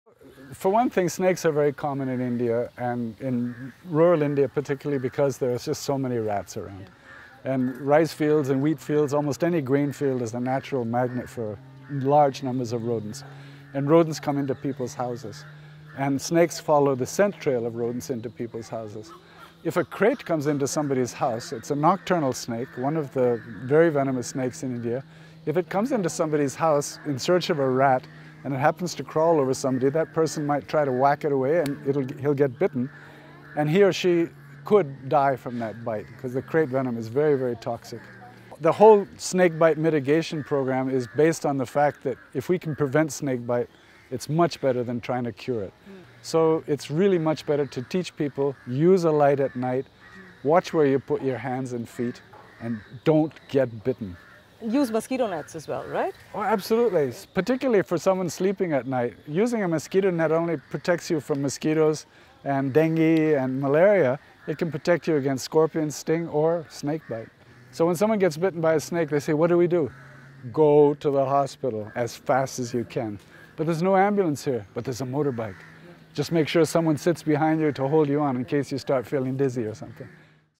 Meet Snake Man of India: Romulus Whitaker | Snake Species | @RoundglassSustain
Do you know how to cope when somebody gets a snakebite?

Watch this and wrap up Snake Week with a grand salute to legendary herpetologist Romulus Whitaker, who shared his incredible insights on the various snake species found across India. Learn how to stay safe from snake bites, and more importantly, what immediate steps to take if bitten — including how to prevent venom from spreading through the body.